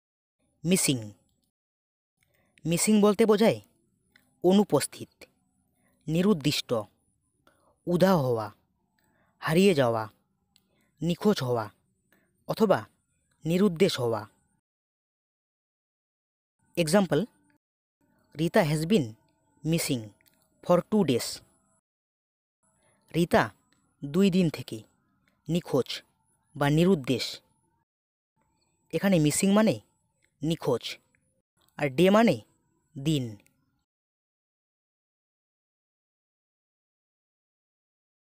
Missing Meaning in Bengali || Missing শব্দের বাংলা অর্থ কি || Bengali Meaning Of Missing
এই ভিডিওতে missing শব্দের বাংলা অর্থ নিয়ে আলোচনা করা হয়েছে।

missing শব্দের বাংলা অর্থ কি?